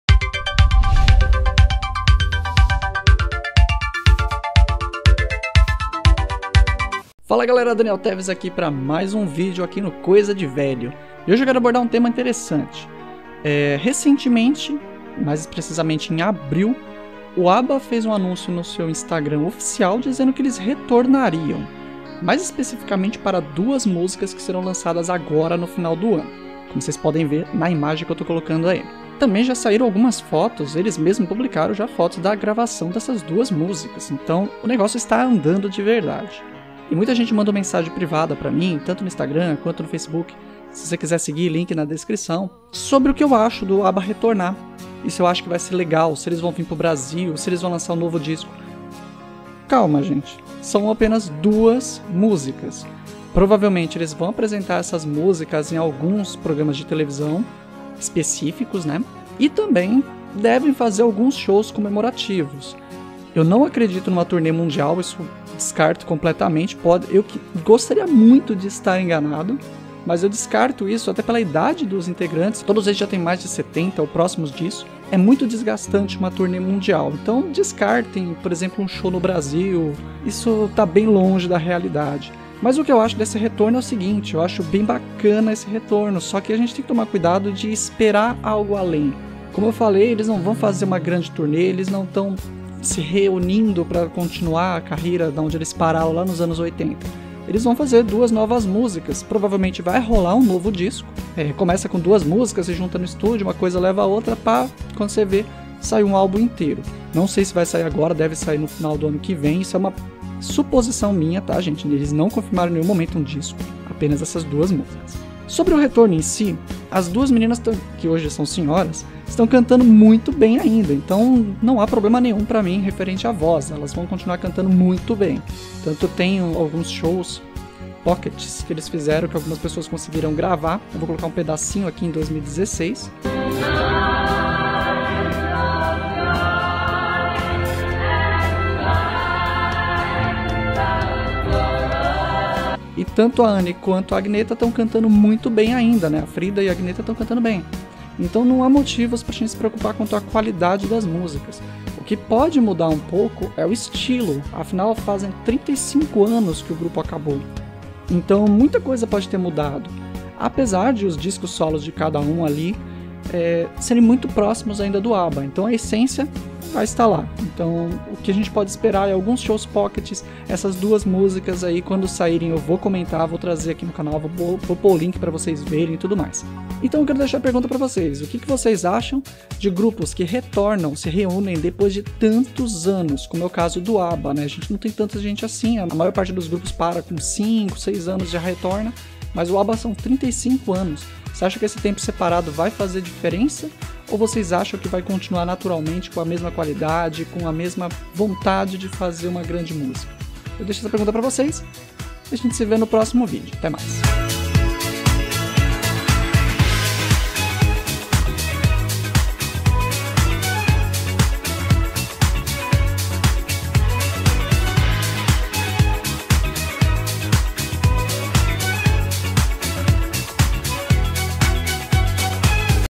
O Retorno do ABBA - Vlog
O Abba anunciou oficialmente o seu retorno em Abril de 2018, esse retorno já está acontecendo, pois já estão gravando as músicas novas com previsão de lançamento para o fim desse ano ainda, confira mais informações e minha opinião no vídeo.

Vídeos as Terças,  Quintas, Sextas e Domingos as 18 horas.

Grupo do Canal no Facebook, onde todos podem compartilhar e conversar sobre os anos 80 e 90: